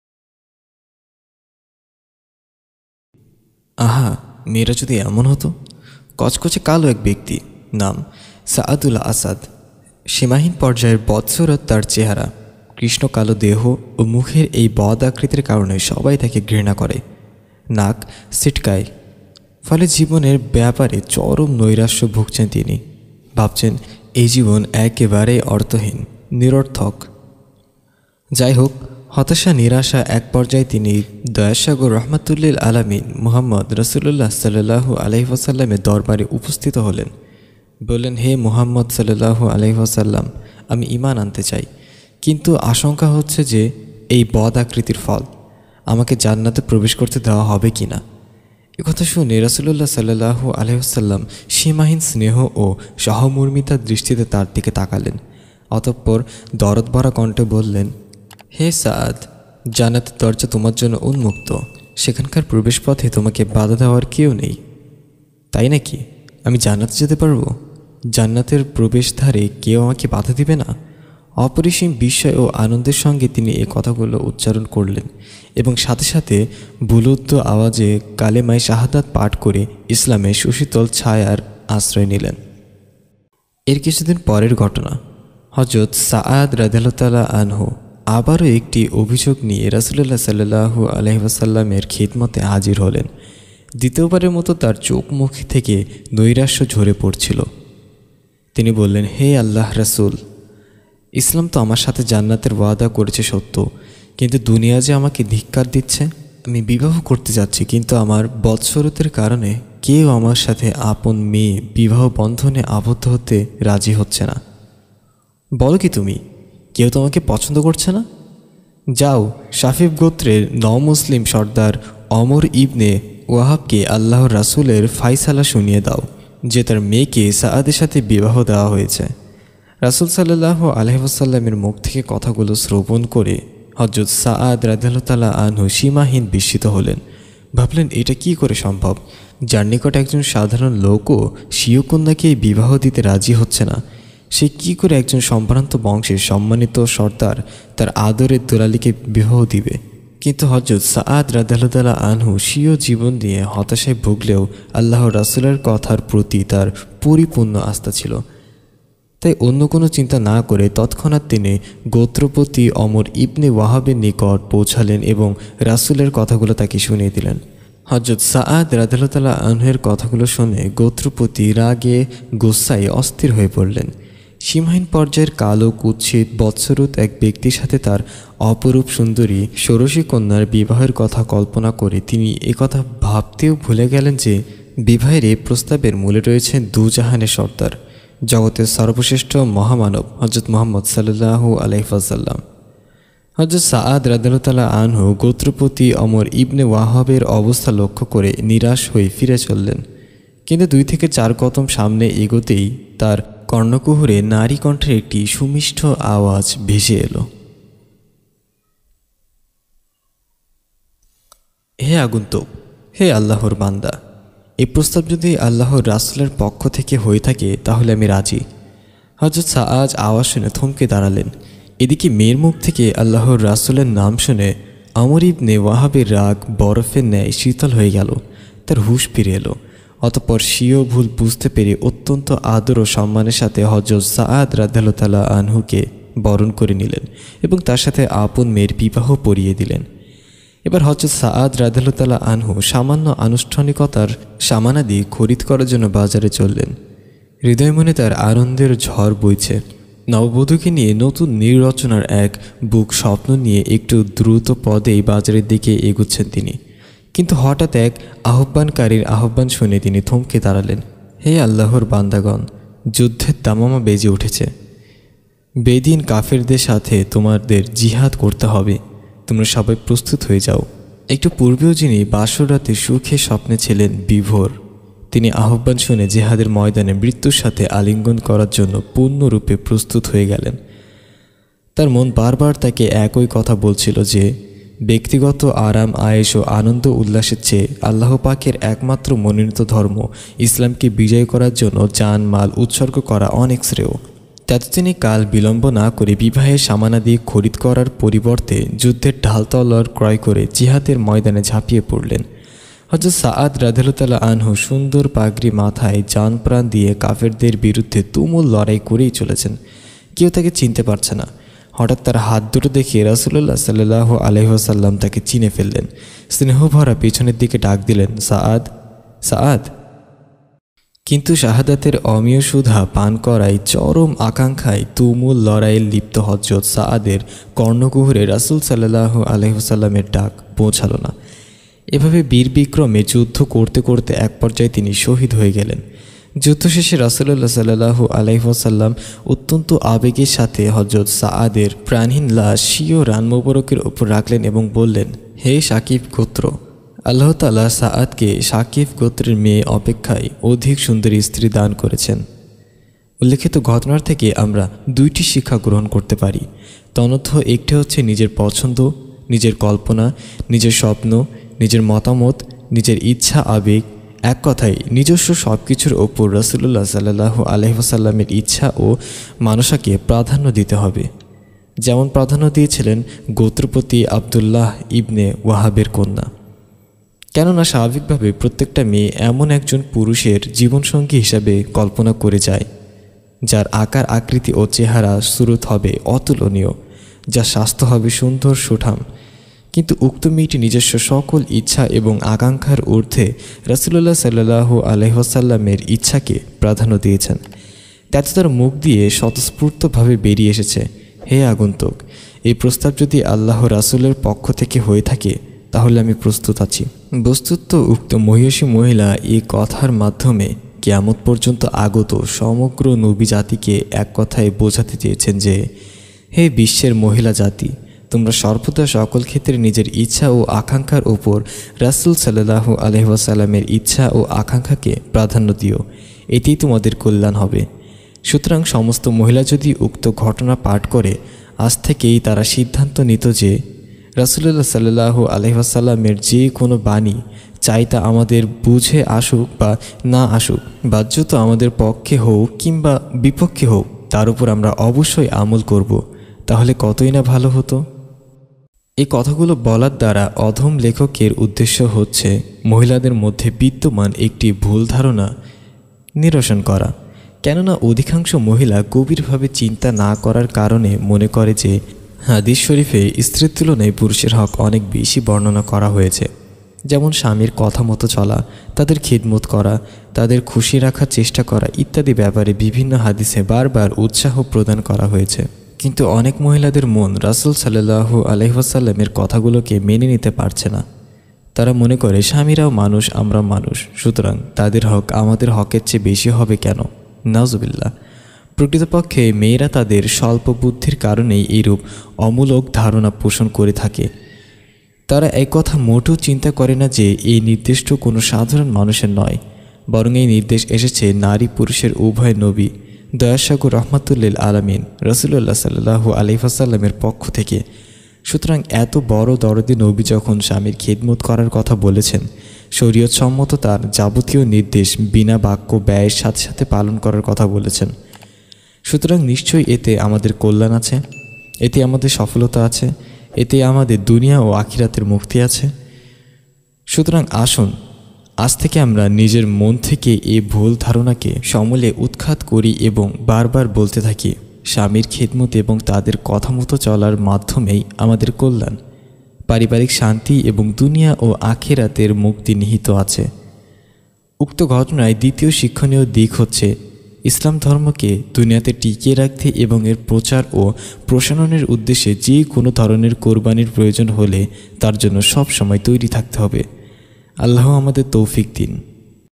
বিশ্বনবী (সাঃ) এর কালো সাহাবীর বিয়ের ঘটনা | biography of companions
Md Mohiuddin & Madina Series presents 'Md Zafor Hossain"বিশ্বনবী (সাঃ) এর কালো সাহাবীর বিয়ের ঘটনা | biography of companions.